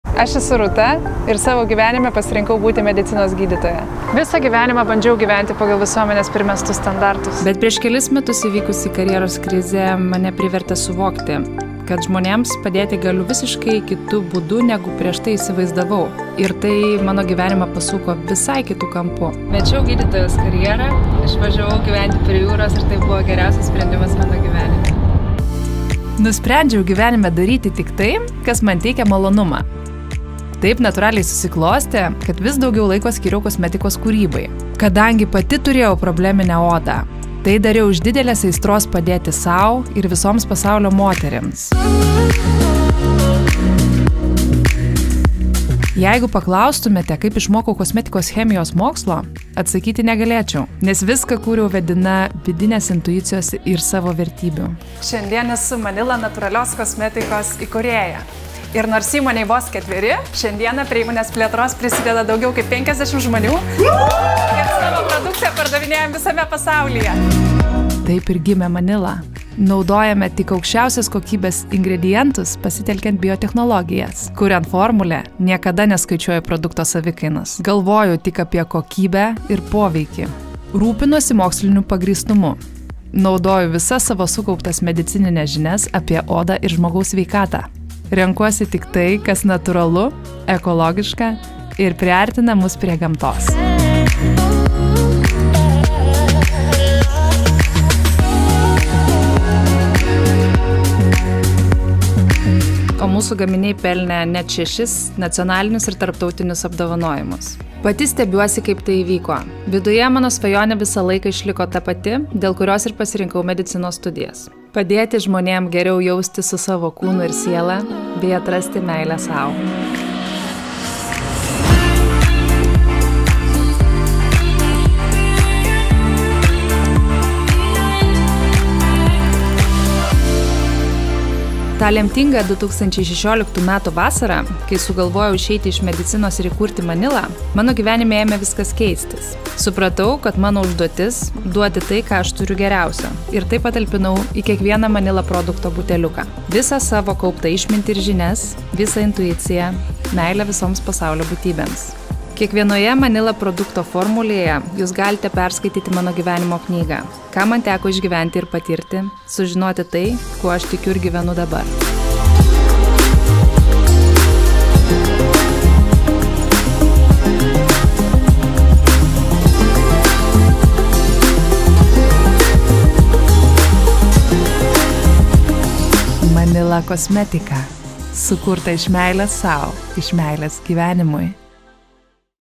MANILLA – sukurta iš meilės gyvenimui!
MANILLA gimė 2016 – aisiais metais ir per šiuos ketverius metus įvyko tiek daug, kad mums patiems kartais užgniaužia kvapą! ⁠
Nuo to laiko sukūrėme virš 10 odos priežiūros priemonių linijų, kurios buvo įvertintos net 6 apdovanojimais, įkūrėme 5 parduotuves ir šiuo metu mūsų komandoje bei prie įvairių projektų dirba daugiau nei 50 žmonių! Tačiau svarbiausia, kad atradome savo klientą – Jus, kurie esate mūsų kasdieninė varomoji jėga kurti, tobulėti ir nėra nieko maloniau už žinojimą, jog mūsų produkciją vertinate ir ja pasitikite. ⁠
Mes sakome ačiū Jums ir kviečiame dar geriau susipažinti!⁠ ♥️